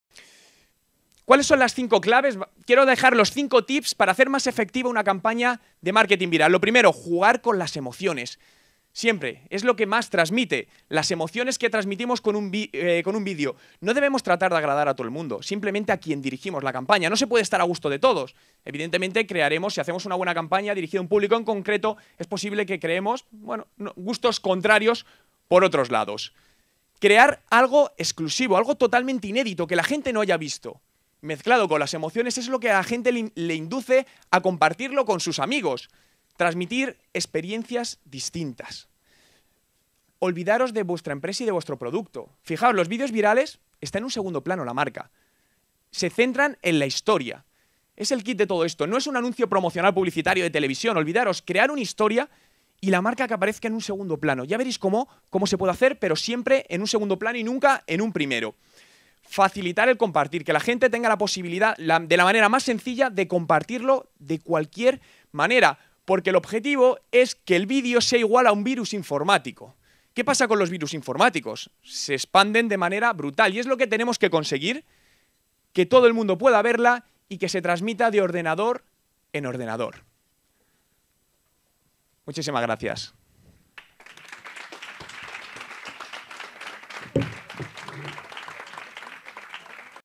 5 claves para mejorar la viralidad
Juan Merodio nos da cinco pistas clave para incrementar la difusión viral de un video. En la primera edición de "De Youtube a Entre Copas", el 25 de Noviembre de 2011, en Madrid